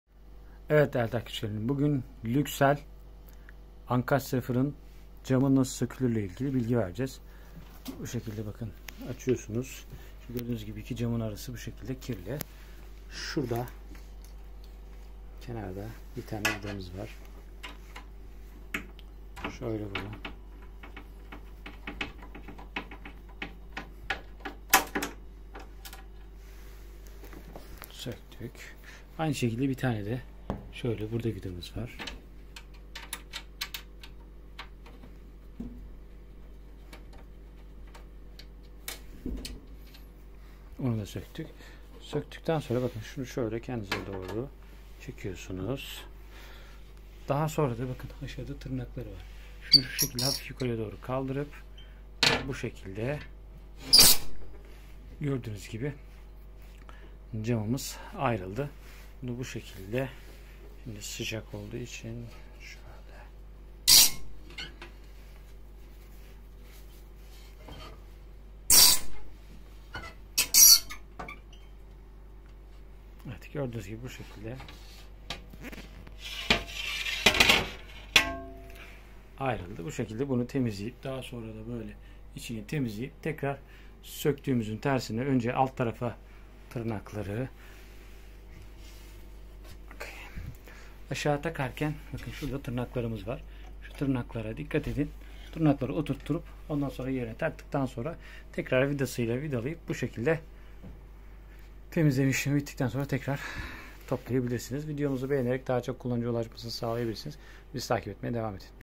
Luxell Ankastre Fırın Çamı Nasıl Sökülür / Luxell Fırın Camı Nasıl Temizlenir /
Luxell Ankastre Fırın Çamı Nasıl Sökülür ilk olarak kapağı açın. Sağ ve solda bulanan iki vidayı sökün camı yerinden çıkartın. Temizleme işlemi bittikten sonra tekrar söktüğünüz gibi yerine takın Luxell Fırın Camı Nasıl Temizlenir hakkında bilgi verdik.